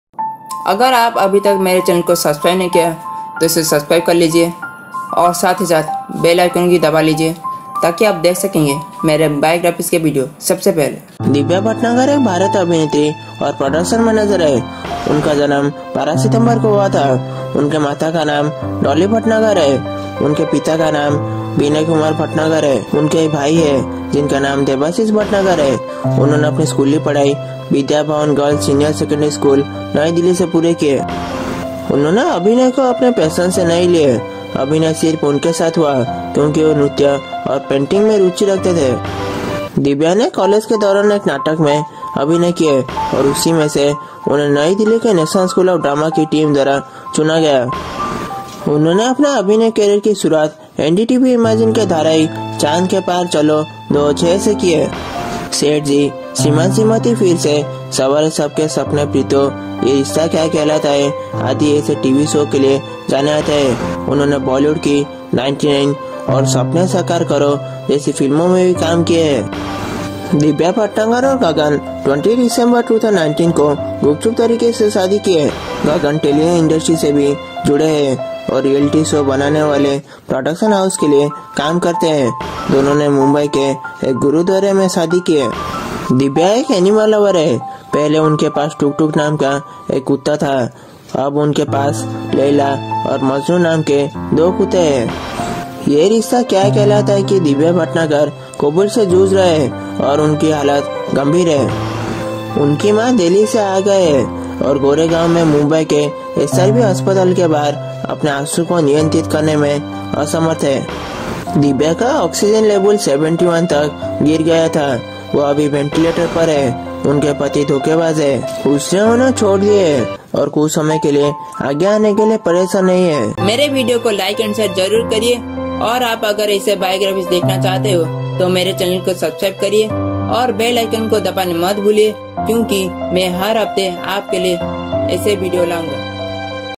Divya Bhatnagar Biography | Lifestyle,Life Story,Wiki,Interview,Serials,in yrkkh,Husband,Wedding,Age
DivyaBhatnagar did not take acting desirably, acting just happened to her she was instead interested in dancing & painting. Divya performed in a drama play during college and from that she was selected by the team of National School of Drama, New Delhi.She debuted her acting career with the NDTV’s serial Chand Ke par Chalo (2006).